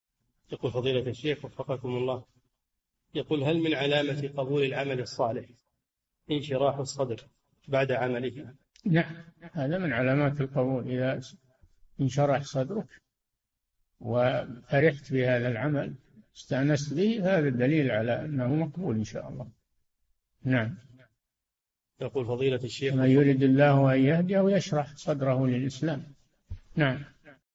[4254- 4717] هل من علامات قبول العمل الصالح انشراح الصدر بعده؟ - الشيخ صالح الفوزان
"[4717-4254] هل من علامة قبول العمل الصالح انشراح الصدر بعد عمله؟
الجَوَابُ: نعم هذا من علامات القبول إذا انشرح صدرك وفرحت بهذا العمل واستأنست به فهذا دليل على أنّه مقبول إن شاء الله، ﴿فَمَنْ يُرِدِ اللَّهُ أَنْ يَهْدِيَهُ يَشْرَحْ صَدْرَهُ لِلْإِسْلَامِ﴾ [الأنعام : 125]

للاشتراك في قناة الشيخ صالح الفوزان
فتاوى المجالس في تفسير المفصل

للاستفسار والإقتراحات والدعم
رئيس الفريق
لمشروع كبار العلماء